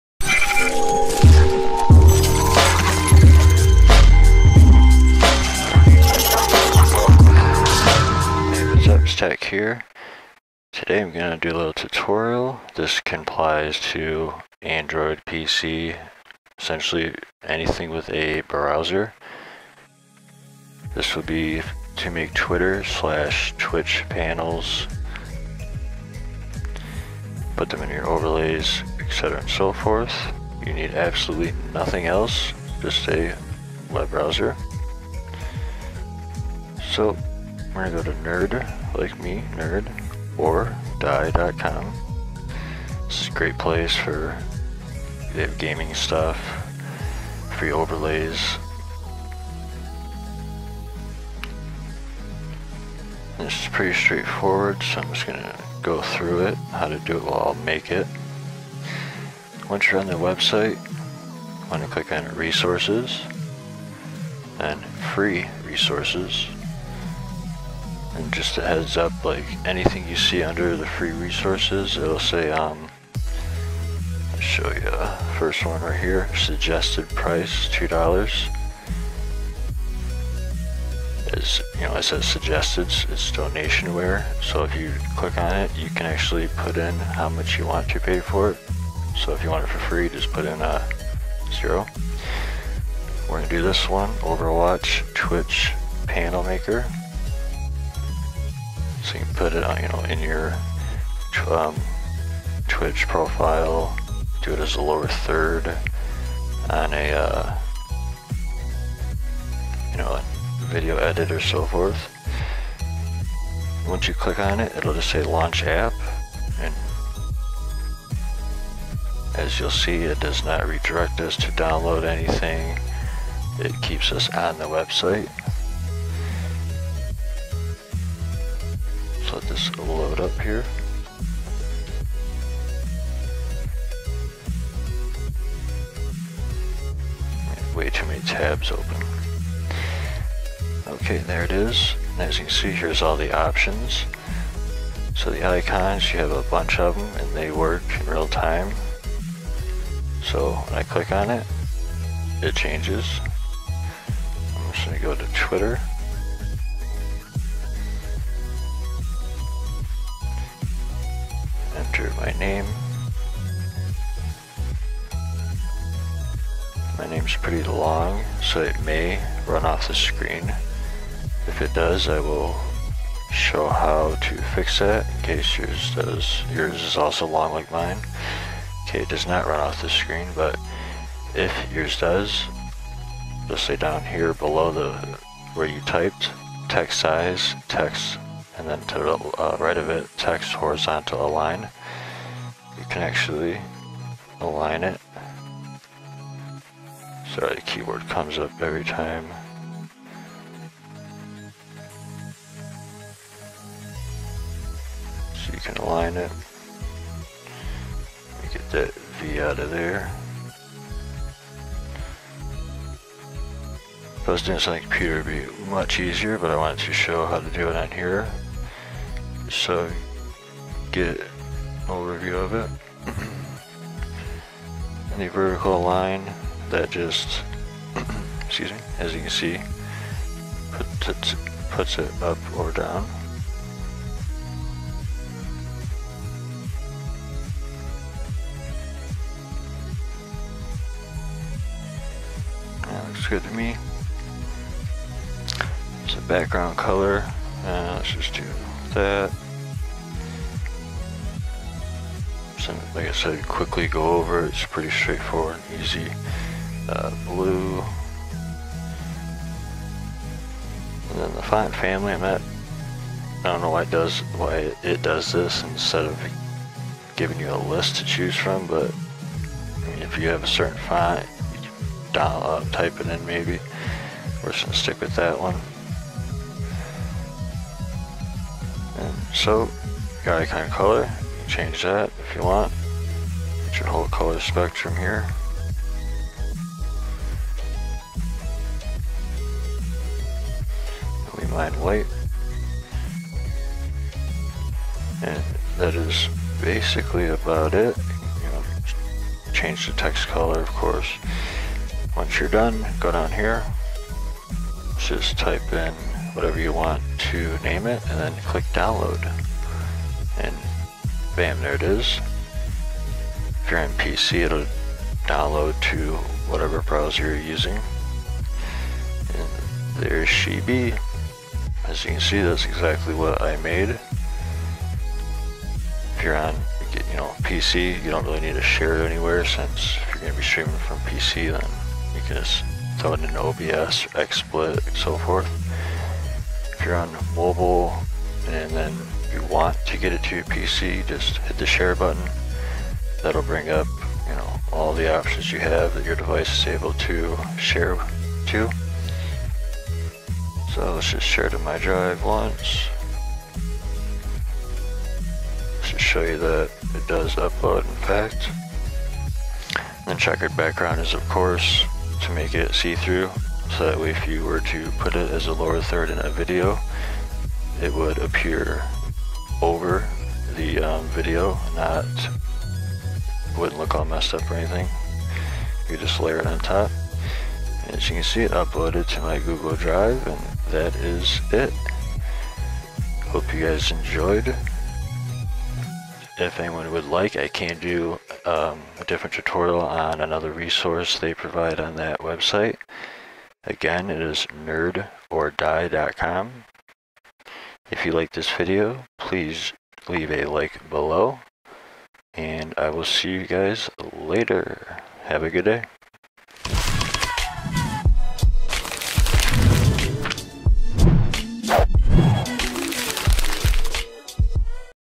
How To Make Twitch, Twitter Panels on Mobile and PC EASY
I Show How you Can Make You Own Custom Twitch and or Twitter Panels, Overwatch Themed GFX. I Do This on My Tablet to Show how easy it Actually is, Works For Android, IOS, PC, PC. Make Easy Overlay / Profile Panels on Mobile or PC

Site: Nerdordie.com